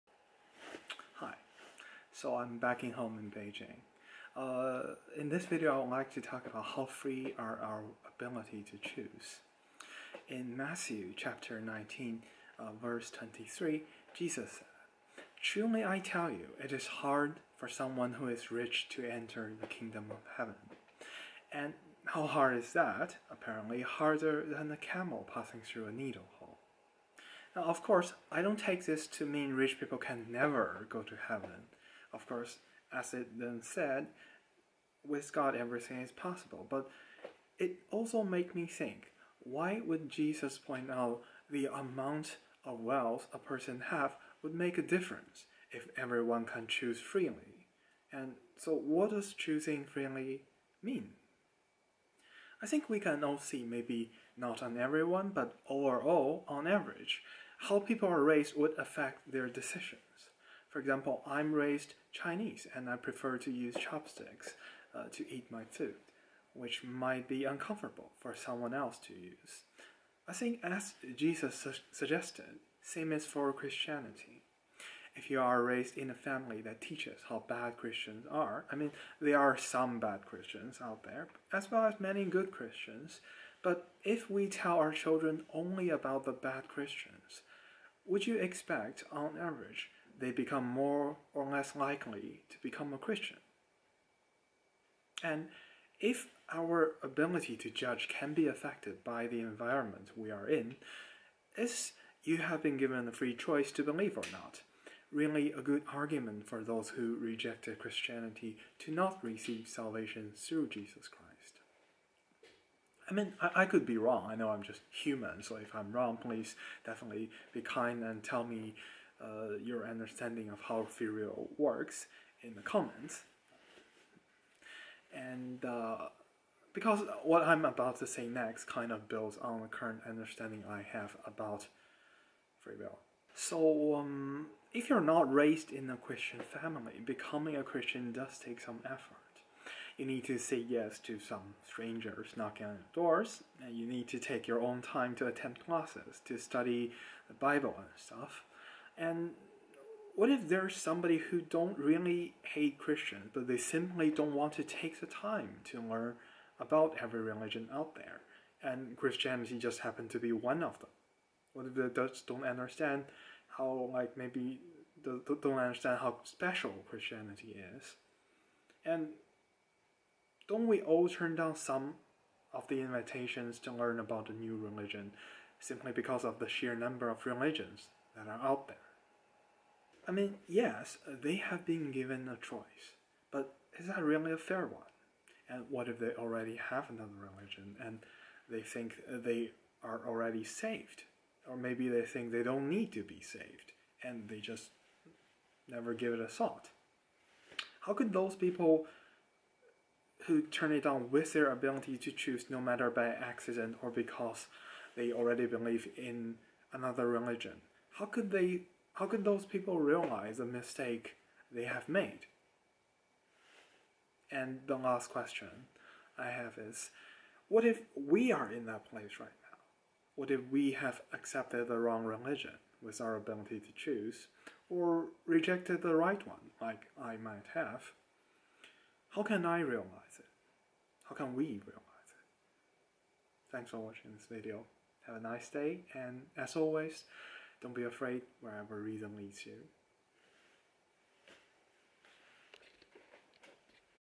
Dear Christians: How Free are Wills?
If our decision on which religion to join is affected by our experiences, to what extent do we have free wills?

This is my attempt to discuss the topic of free will in a faith-friendly way, also without invoking bleeding-edge physics or neuroscience that most of us don't really understand (as in, have a degree in).